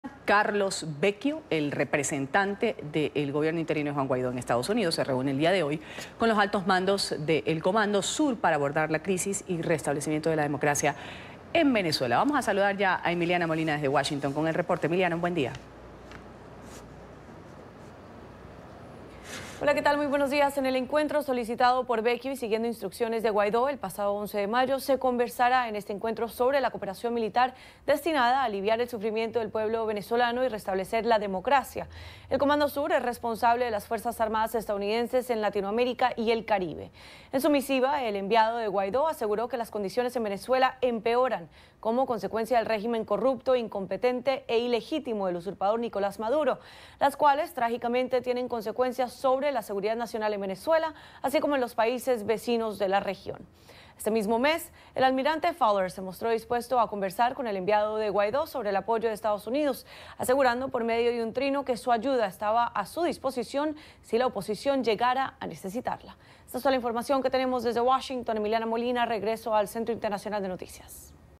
Carlos Vecchio se reúne con altos mandos del Comando Sur de EEUU
Carlos Vecchio, representante del Gobierno del presidente interino de Venezuela, Juan Guaidó, en Estados Unidos, se reúne este lunes con altos mandos del Comando Sur para abordar la crisis y el restablecimiento de la democracia en Venezuela.